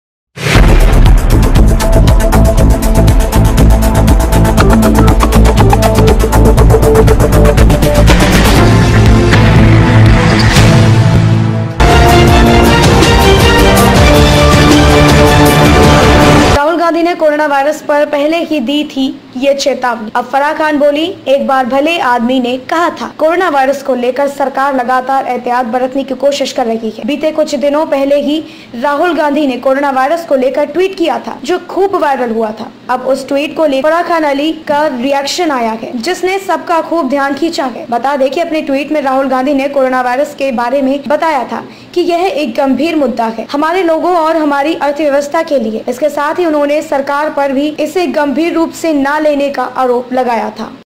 राहुल गांधी ने कोरोनावायरस पर पहले ही दी थी ये चेतावनी...
राहुल गांधी ने कोरोनावायरस पर पहले ही दी थी ये चेतावनी, अब फराह खान बोलीं- "एक बार भले आदमी ने कहा था..."